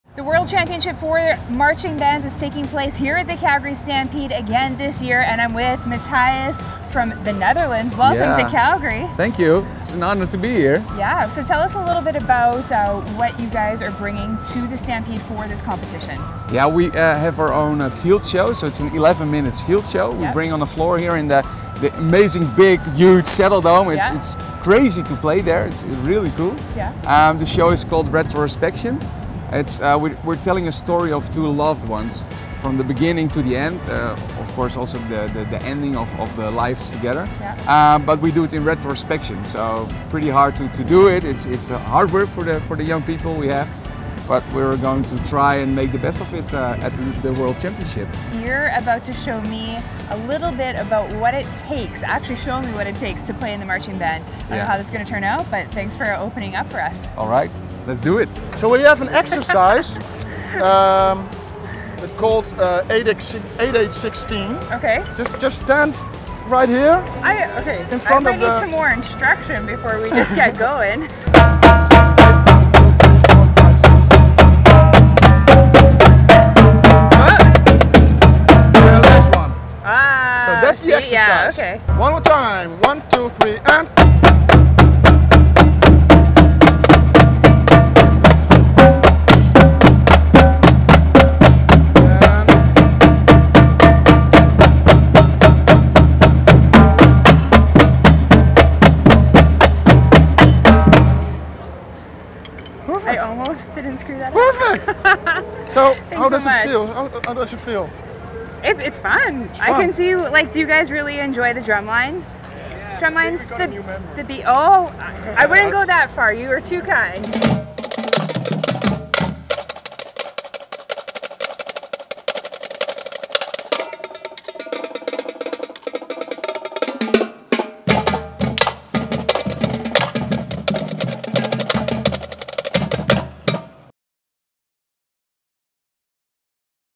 Calgary Stampede Roving Reporter - Showband Marum - WAMSB
From pitch perfect music, spectacular choreography, and unbreakable team spirit, see firsthand what it takes to be a World Champion marching band during Stampede 2019. The Marum Showband all the way from the Netherlands shows Cayla a sneak-peak at what they are bringing to this years competition.